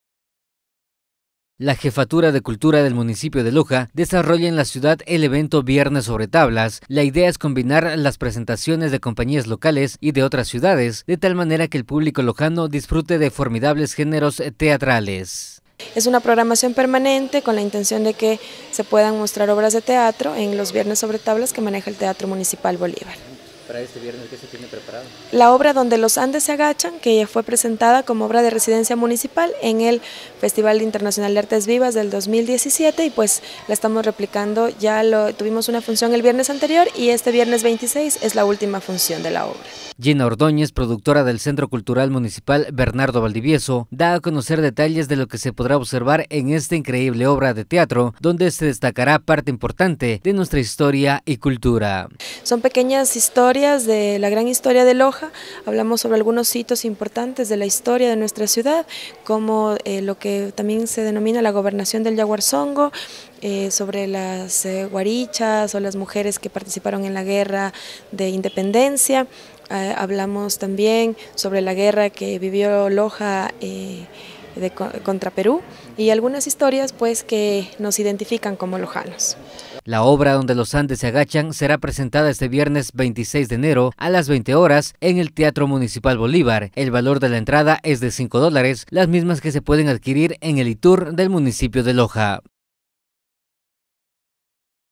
Jefatura de Cultura del Municipio de Loja desarrolla evento viernes sobre tablas
La Jefatura de Cultura del Municipio de Loja, desarrolla en la ciudad el evento viernes sobre tablas, la idea es combinar las presentaciones de compañías locales y de otras ciudades de tal manera que el público Lojano disfrute del espectáculo.